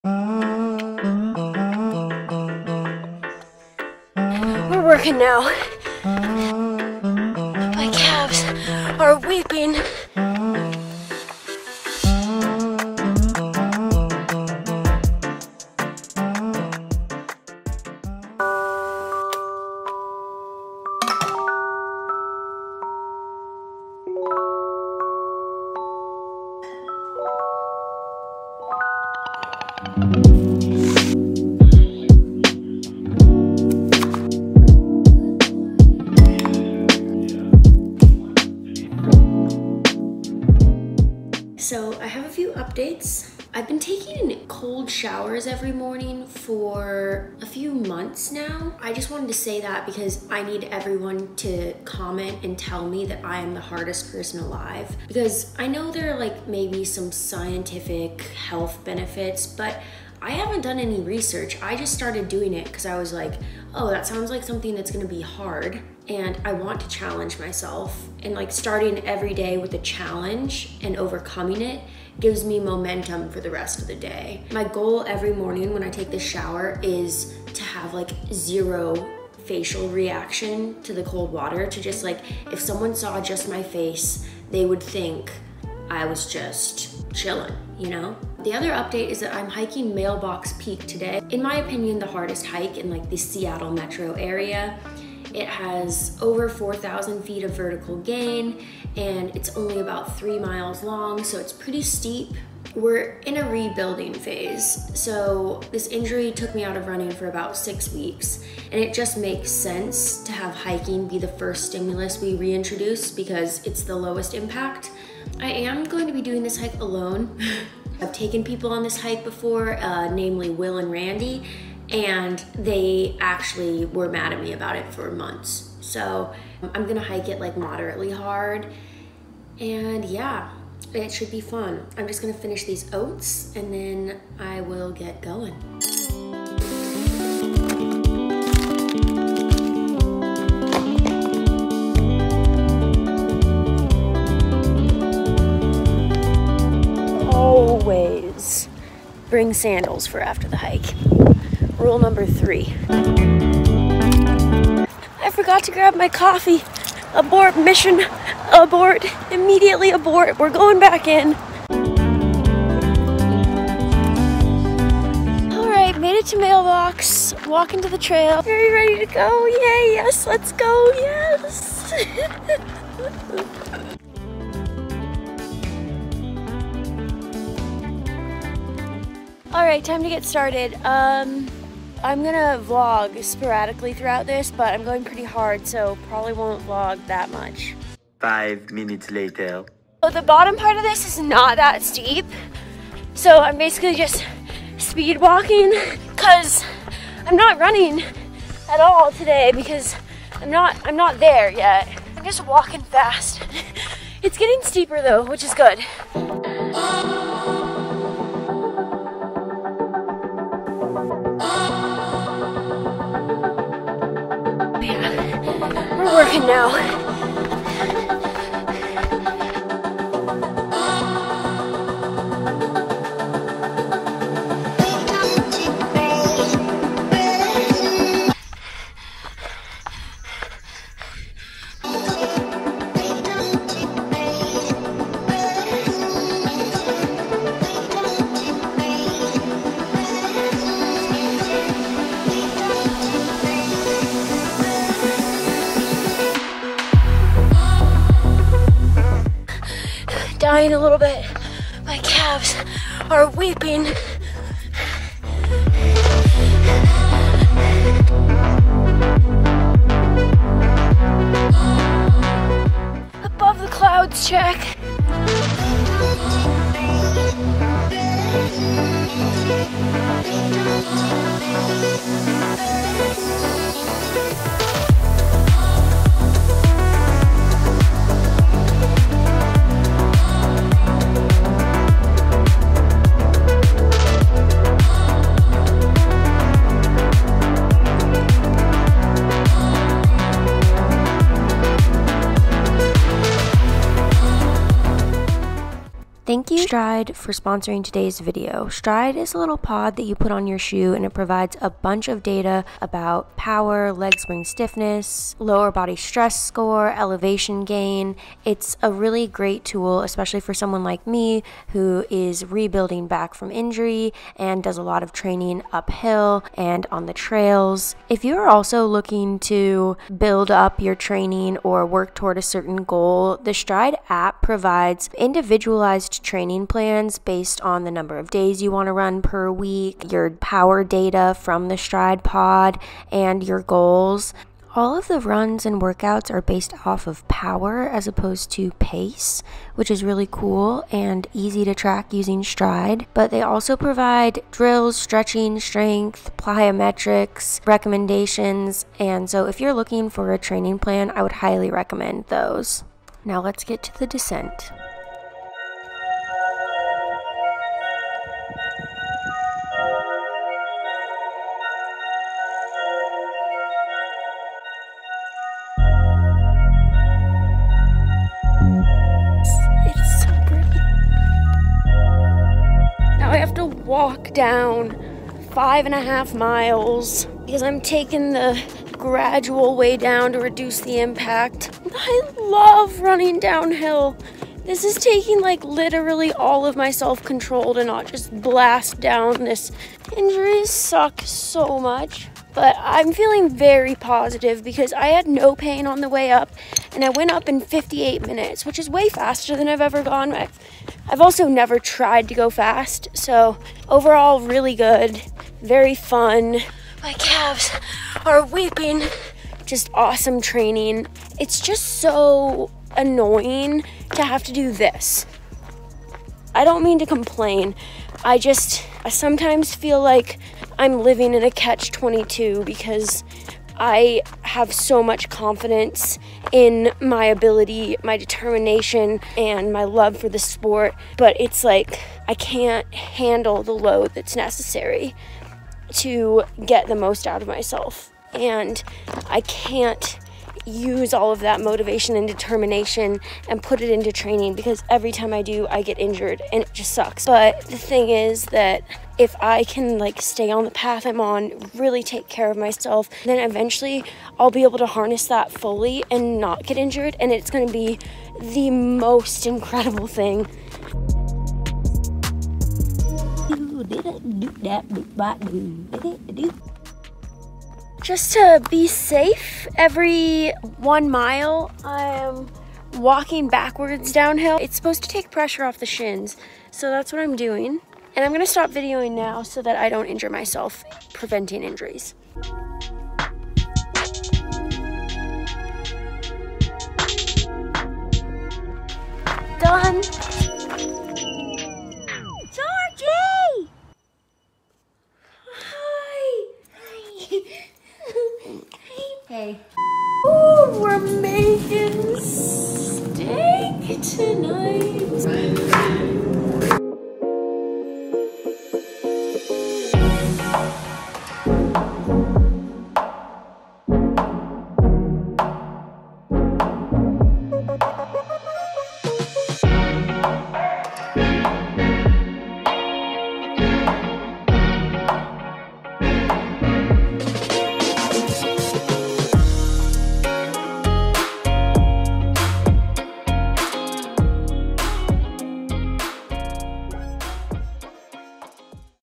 Rebuilding || new goals, first workout back, and training for trails
In this video, I talk about some changes to my week of training, my training schedule going forward, and some of my insecurities and frustrations with training. I also do my first "on foot" workout in 6 weeks! I do a threshold hike up Mailbox Peak, which is one of the hardest hikes in the Seattle Area. The trails near me aren't all that intense or technical, but I think mailbox is honestly incredible training for the European trail races I will do later this summer! Being able to hike and train is giving me new life and I'm finally seeing the light at the end of this injury tunnel!

00:00-00:41 intro, coffee, breakfast
00:41-02:15 training and discipline updates
02:15-03:26 prep and start of hike
03:26-04:55 threshold hike
04:55-05:55 Stryd
05:55-08:12 my biggest struggle
08:12-09:00 dinner and outro

LMNT electrolyte drink to get your free sample pack